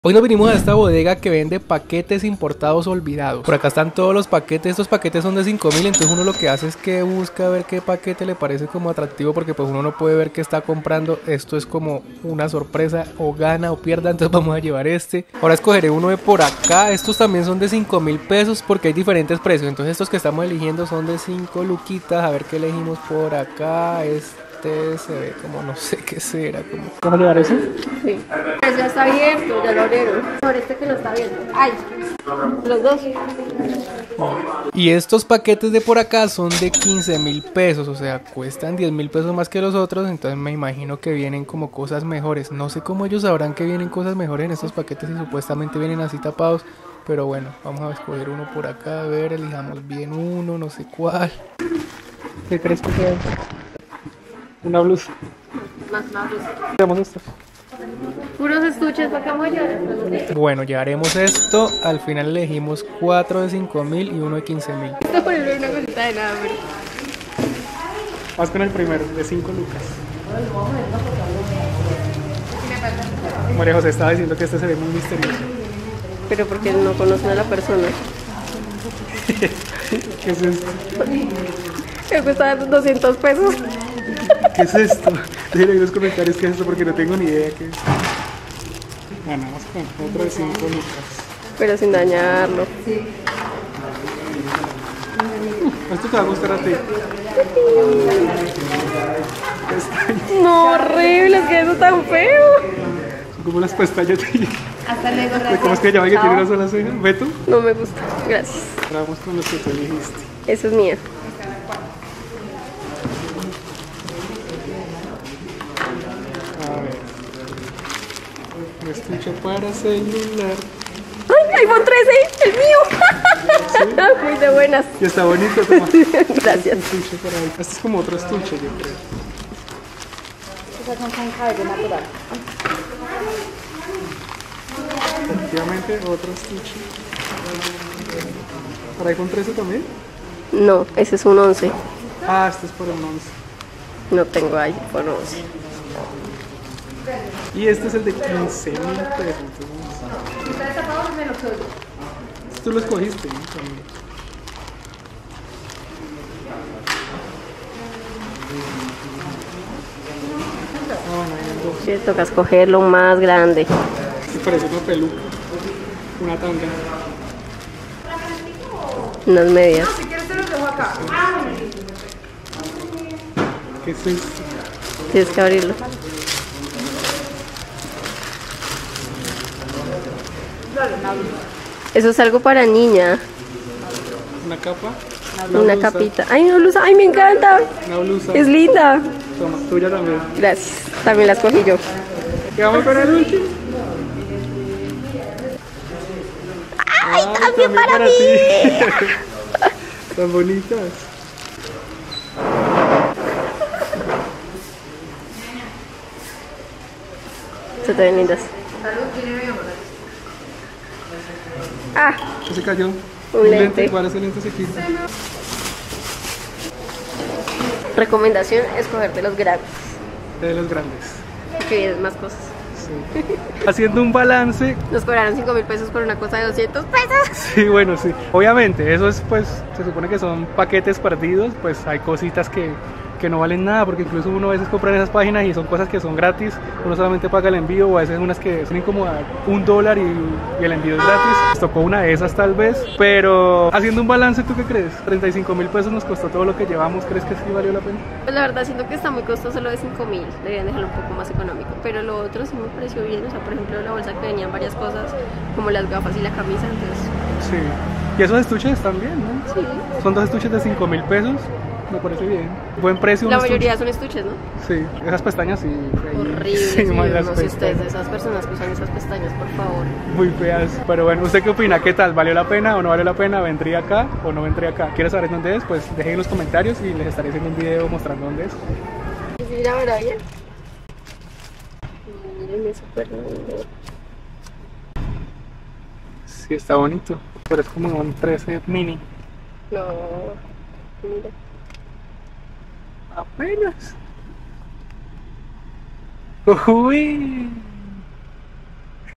COMPRO PAQUETES MISTERIOSOS en Bogotá
En Bogotá hay un lugar donde venden paquetes perdidos, vinimos a comprar algunos a ver que traen dentro, está ubicado en la Calle 12 no 9 69 Bogota centro